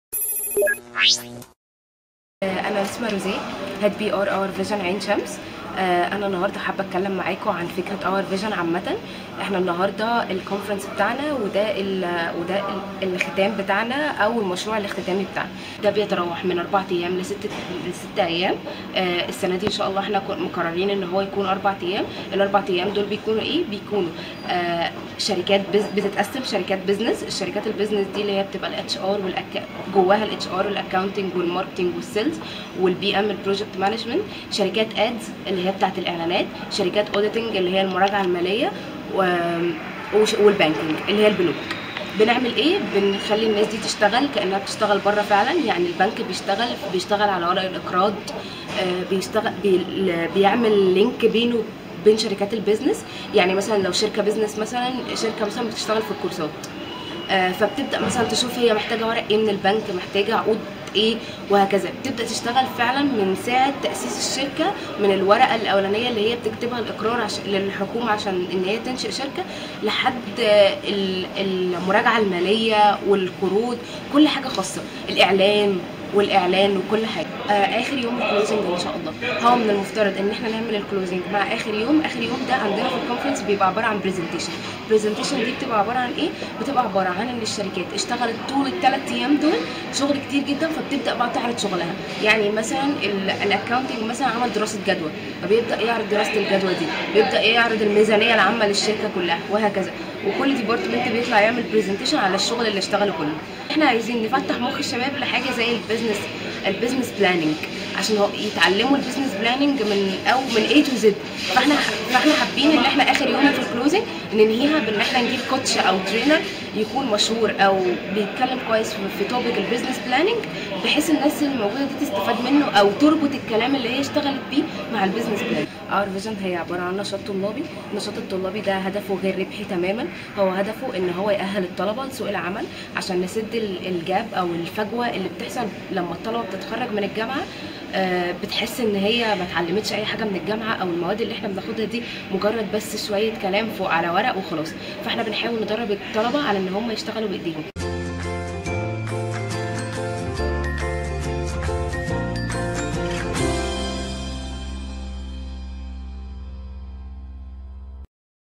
شفاف | "Our Vision" يطلق مشروعه الختامي.. "كمثل حبة"
نموذج "Our Vsion " بجامعة عين شمس يطلق مشروعه الختامي لعام 2016 تحت مسمي "كمثل حبة"

• تصوير: هاجر فهمي
• مونتاچ: مصطفي فارس